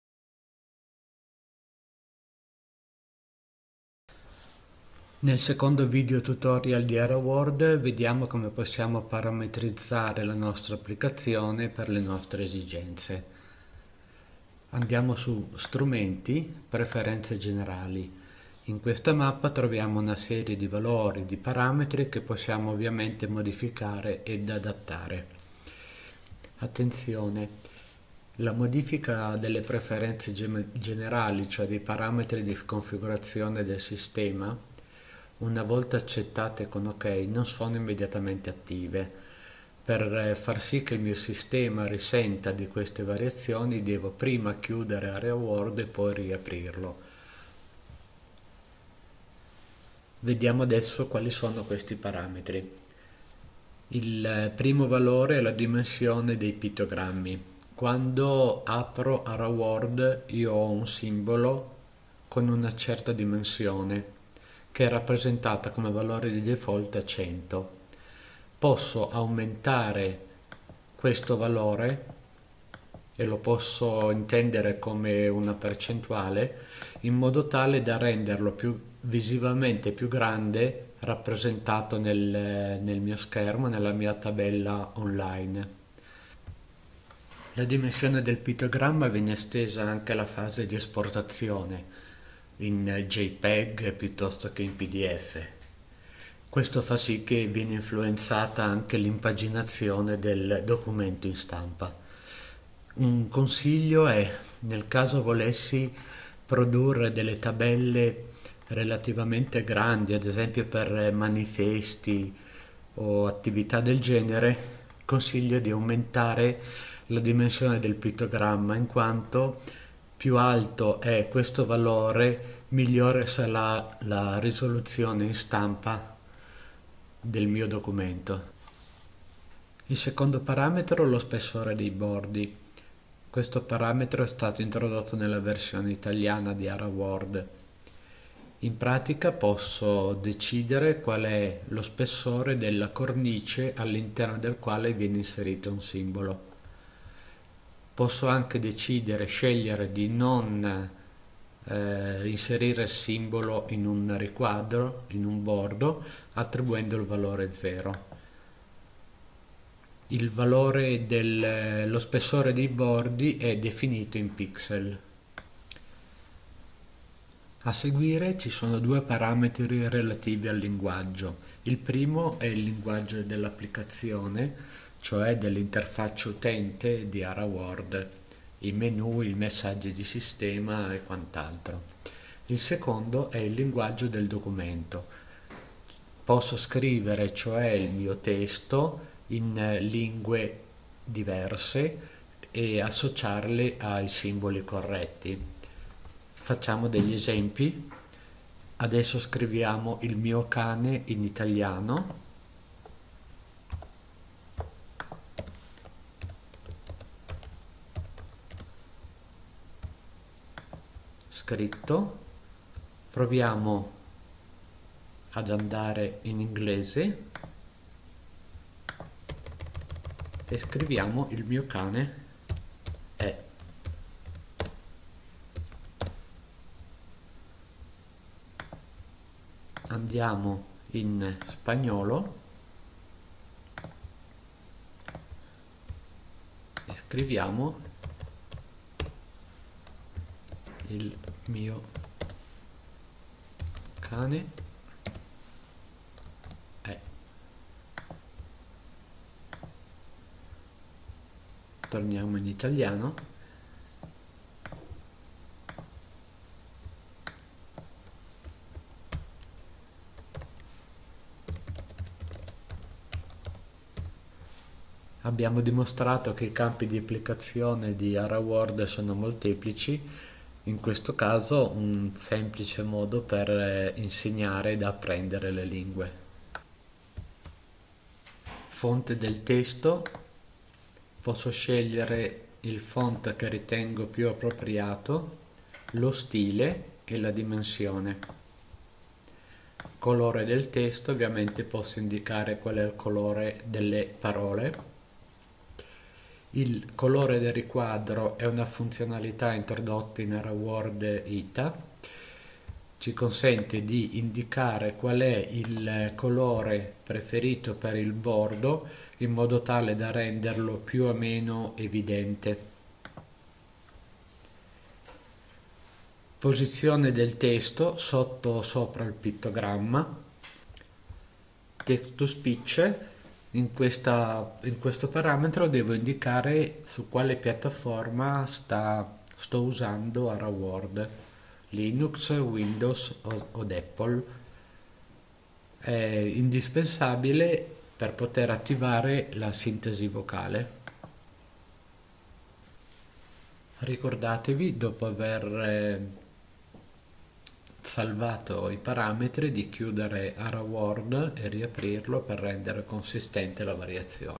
ARAWORD-2- GESTIRE LE PREFERENZE di CONFIGURAZIONE - OSCAR-PASTRONE
ARAWORD APPLICATIVO PER LA CAA- Comunicazione Aumentativa Aumentativa
Video Guida realizzata da Oscar Pastrone del Progetto Radis su
Applicativo per la Comunicazione Aumentata e Aumentativa OpenSource.
Inserito in Sodilinux - sviluppato anche per Windows e MAC